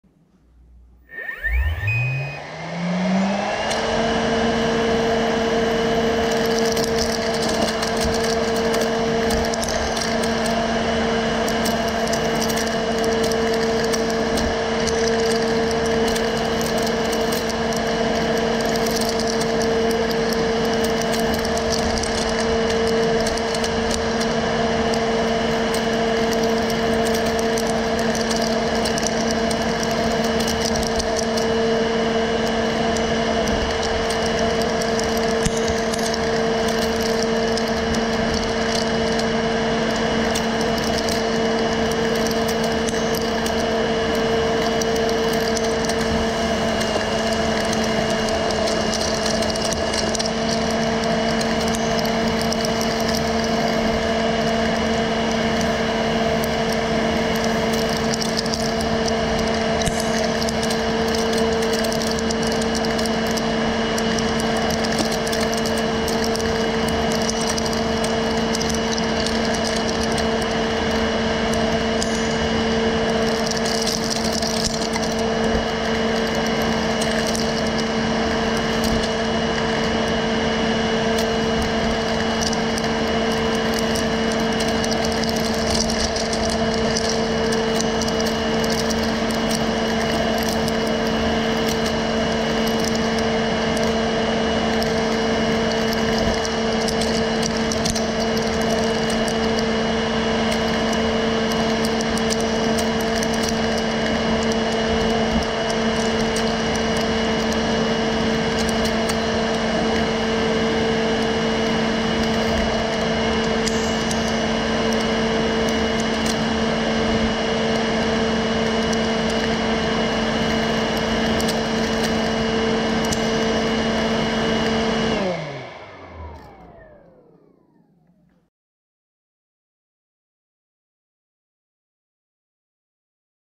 Look inside a working robot vacuum [4K]
Put a GoPro camera inside a robot vacuum and watch how it works.

Robot vacuum